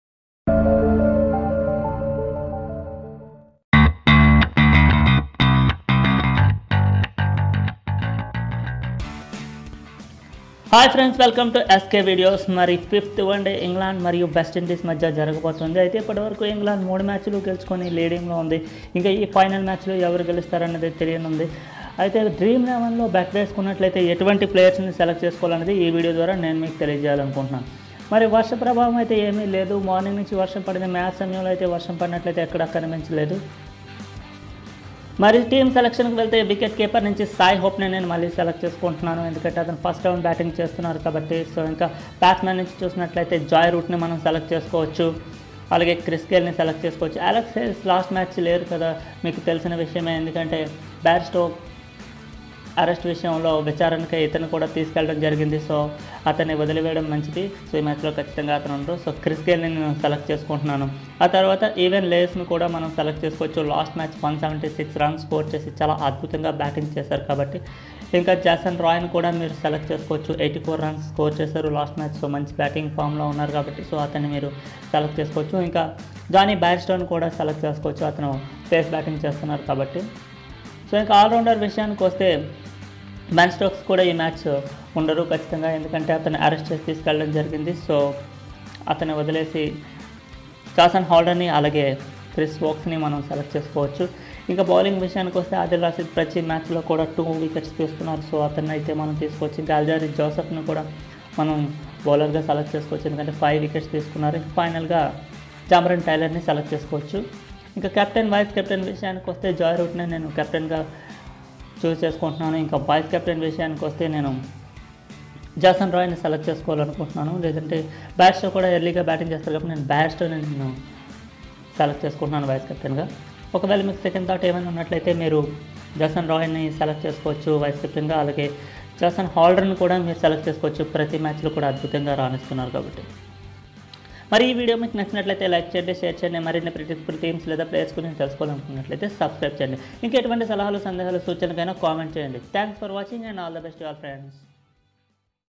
WEST INDIES vs ENGLAND 5th ODI Match prediction or possible team or players
Telugu version Dream 11 fantasy cricket WEST INDIES vs ENGLAND 5th ODI Match prediction or possible team or players.
Series: WEST INDIES tour of ENGLAND 2017
venue: Southampton, England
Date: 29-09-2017

Head to head in this series: WEST INDIES vs ENGLAND (0-3)